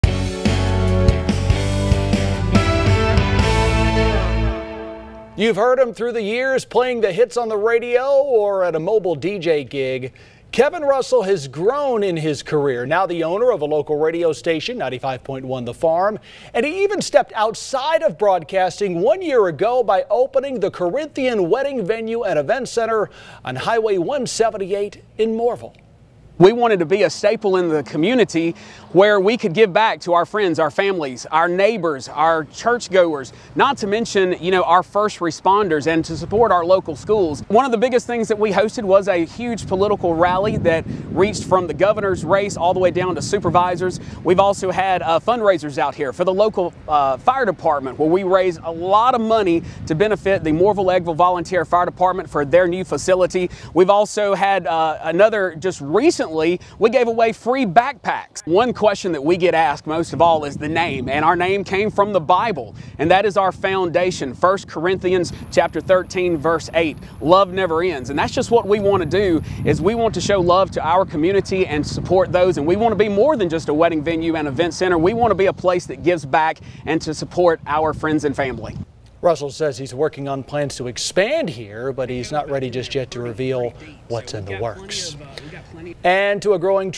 We're Open: The Corinthian Wedding Venue & Event Center in Mooreville
Kevin Russell operates The Corinthian Wedding Venue & Event Center in Mooreville.